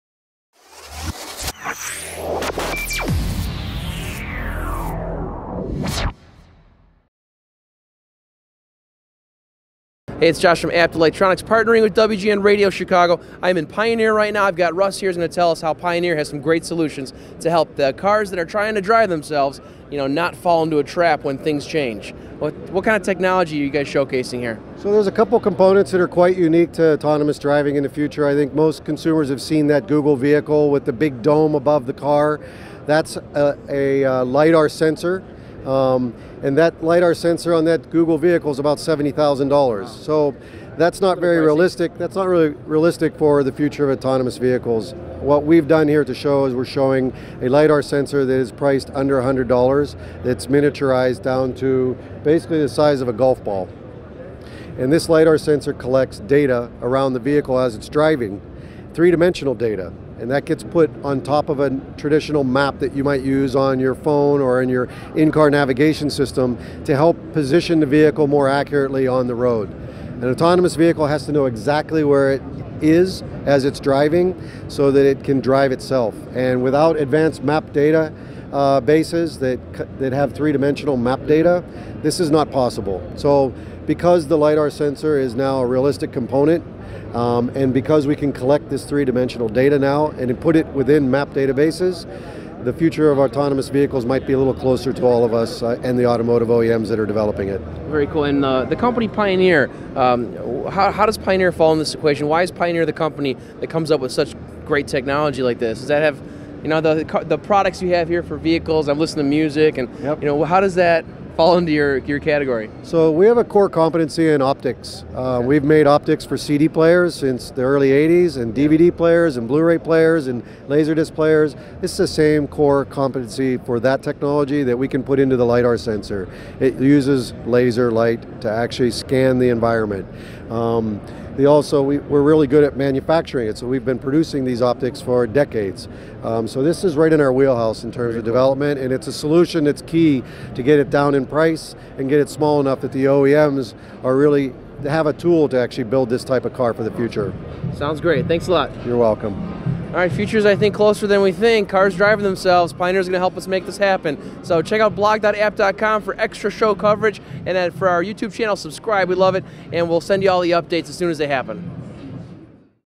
Pioneer LiDAR Sensors and Automated Driving - Abt CES 2016
Abt has all of the latest information on Pioneer LiDAR Sensors and Automated Driving as well as many of the new tech gadgets being shown at CES 2016! For more content visit blog.abt.com today!